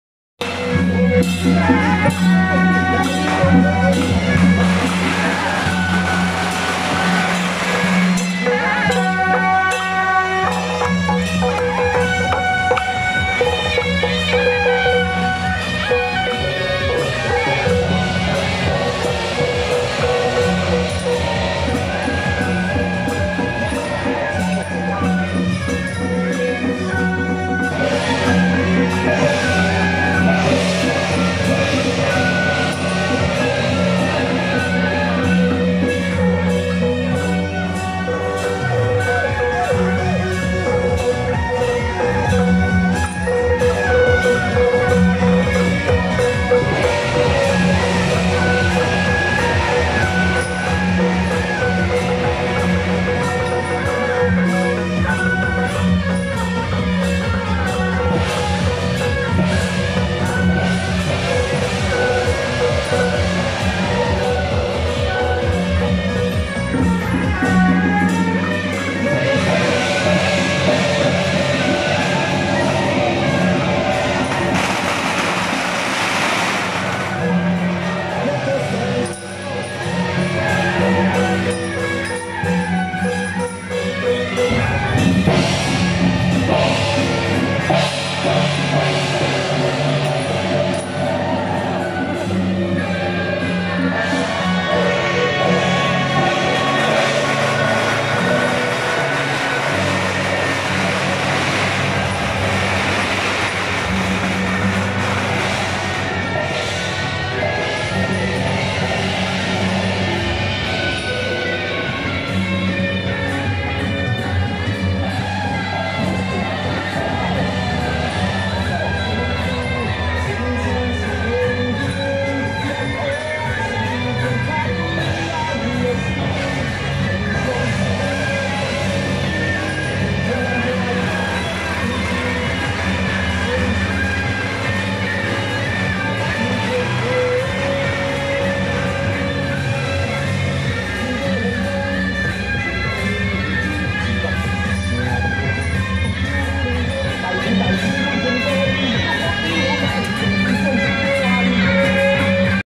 【士林集英社-千順將軍＆士林慈諴宮-列位尊神 聖駕】~110士林慈諴宮恭祝天上聖母聖誕遶境
桃園聖安社-北管＆士林集英社-千順將軍＆士林慈諴宮-列位尊神 聖駕